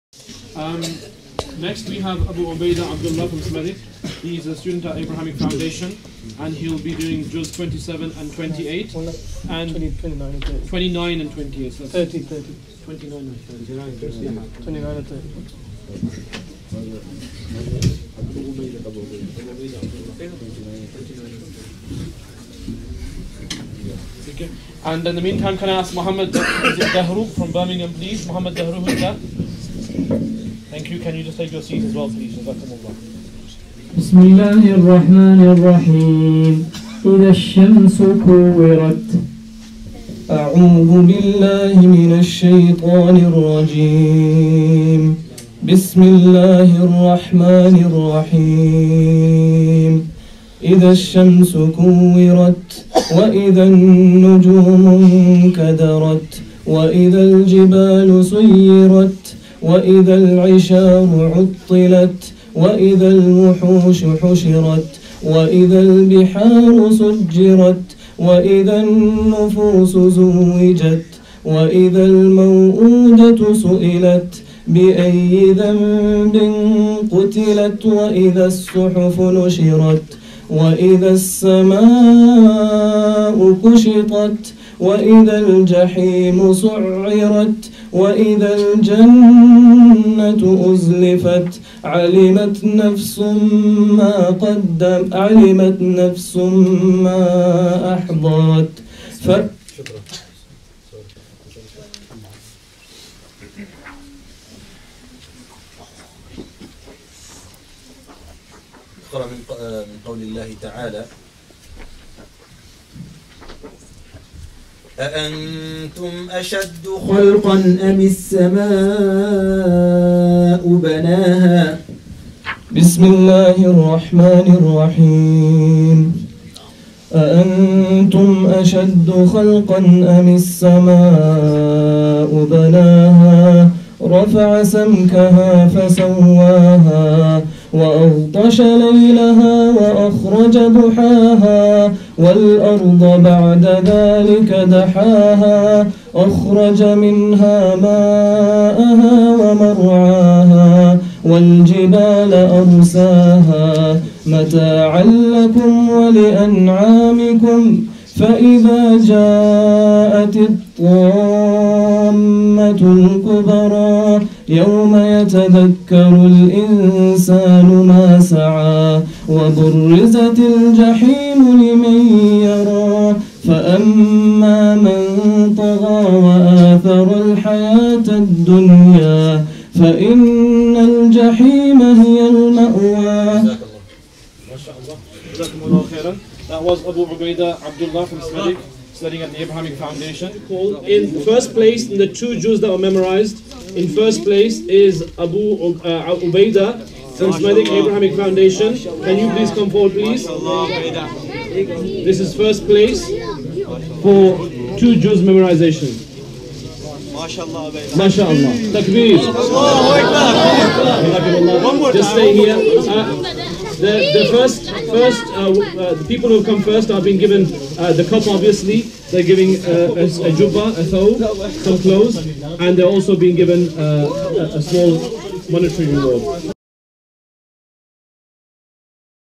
Birmingham Quran Competition 2016 at Masjid Abu Bakr Siddique 2 Juz Category Winner
Birmingham Quran Competition 2016 2 Juz Category Winner Abu-Ubaida Abdullah from Smethwick who studies at The Abrahamic Foundation